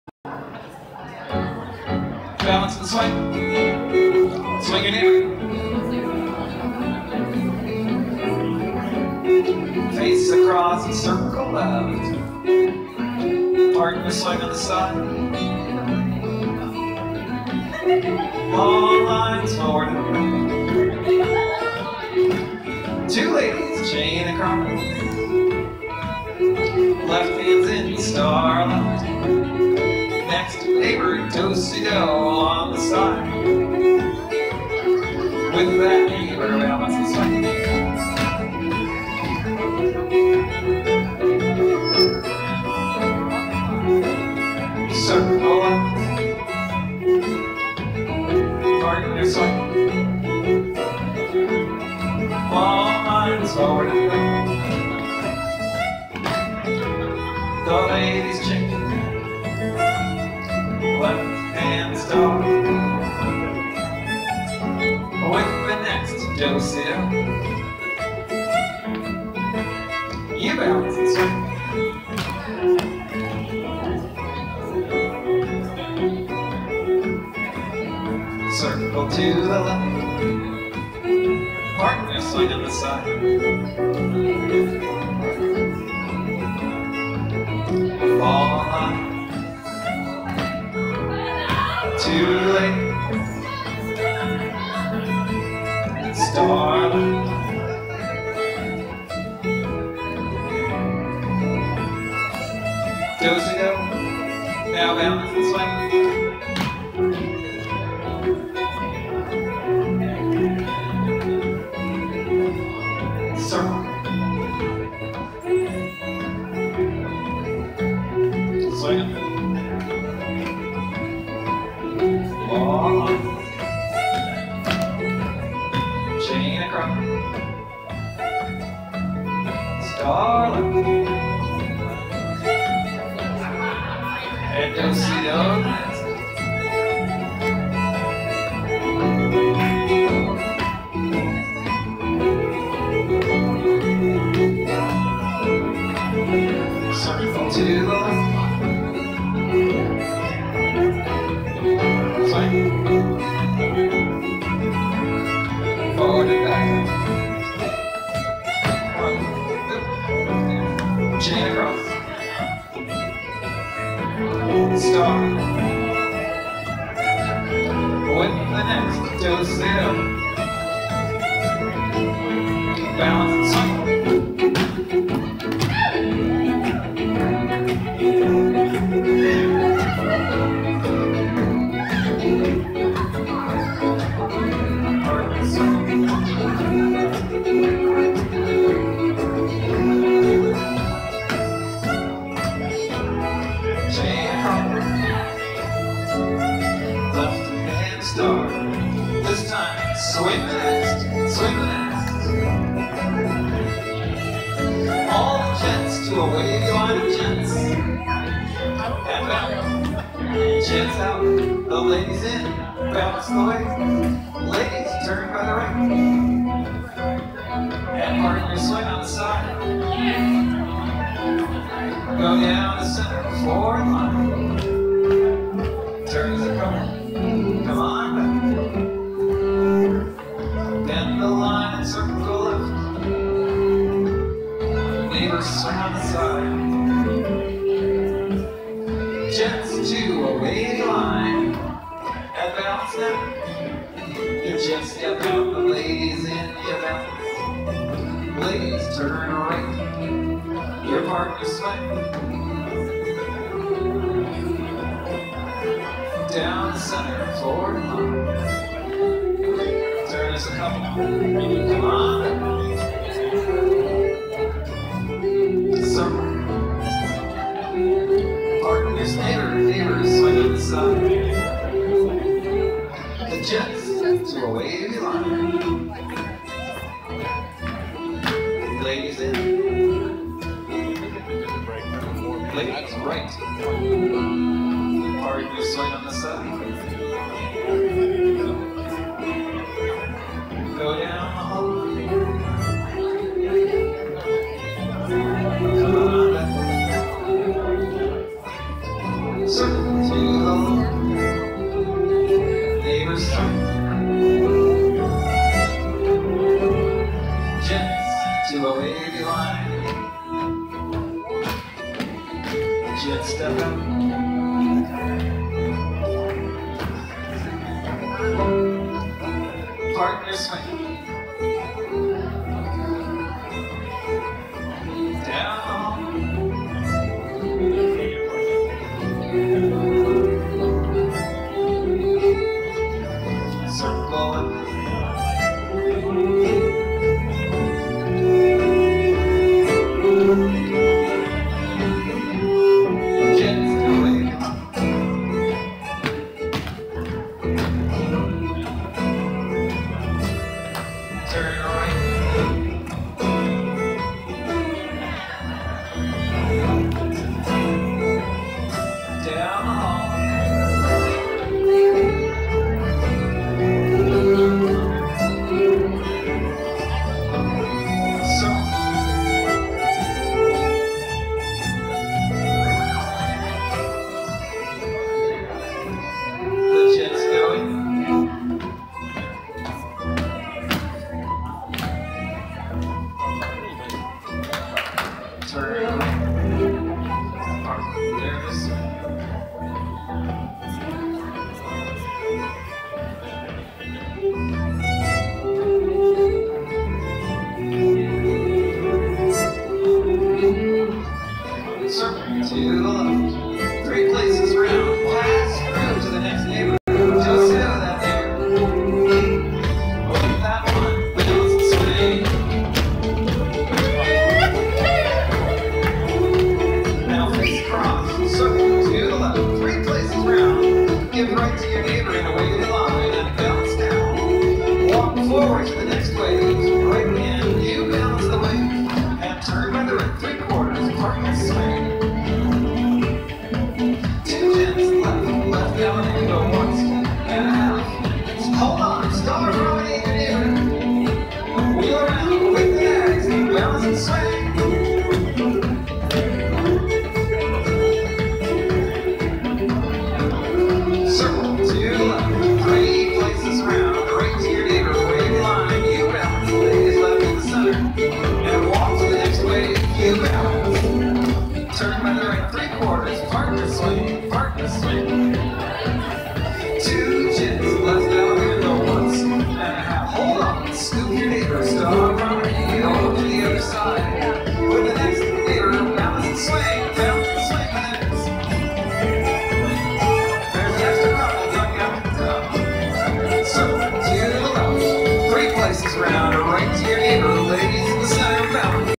Desert Spring Contradance 2018
Rick Mohr calls to Buddy System.